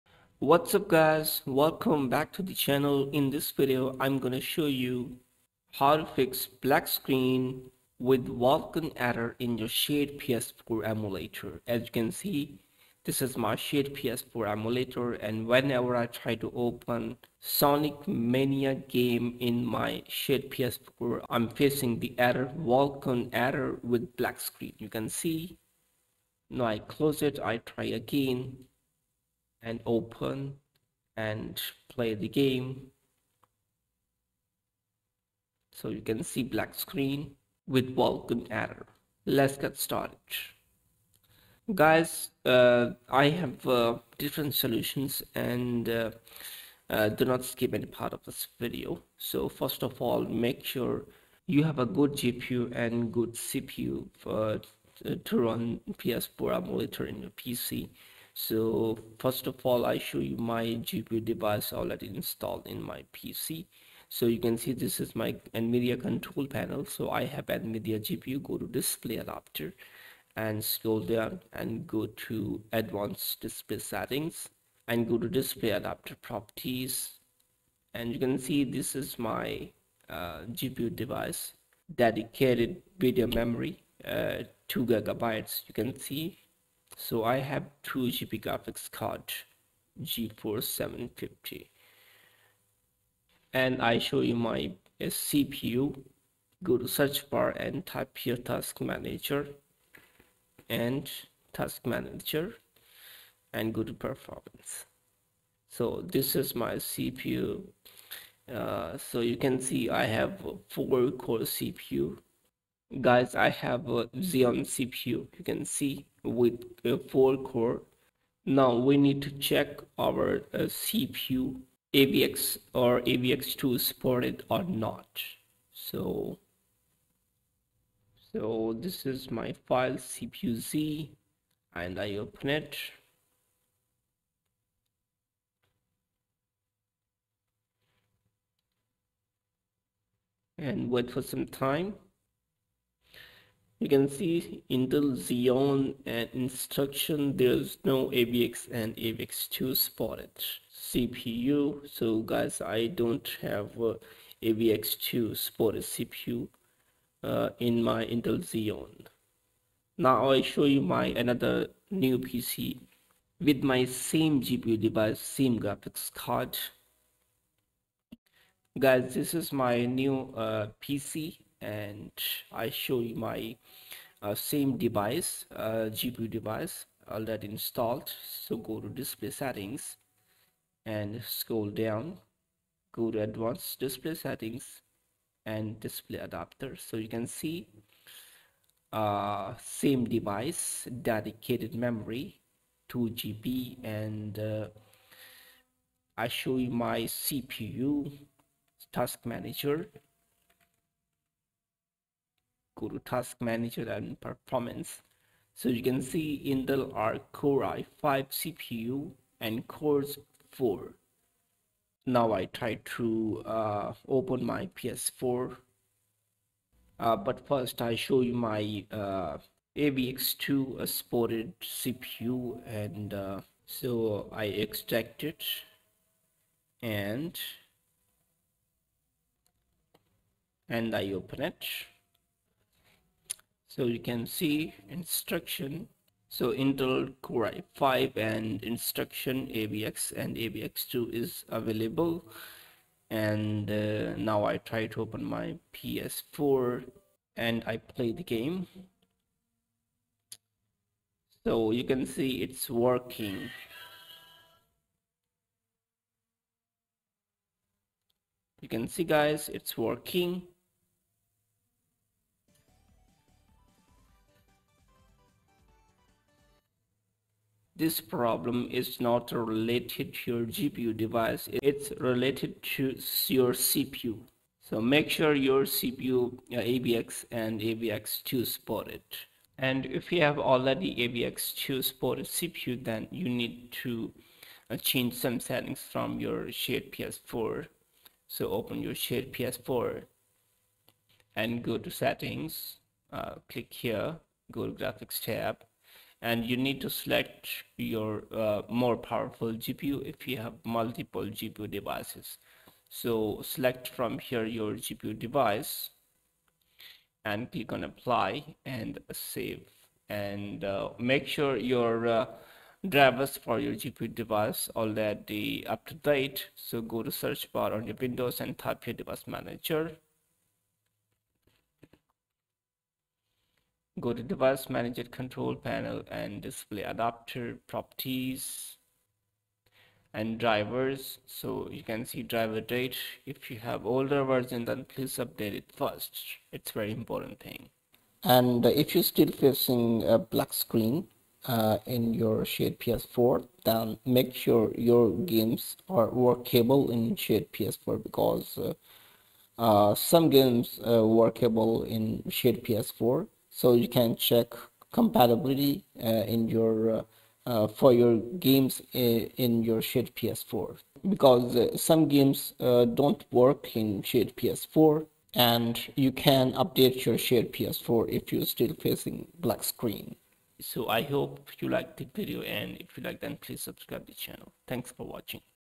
ShadPS4 Emulator Black Screen with Vulkan Error
Hello Guys,
I have shown in this video how to fix shadps4 emulator black screen with vulkan error.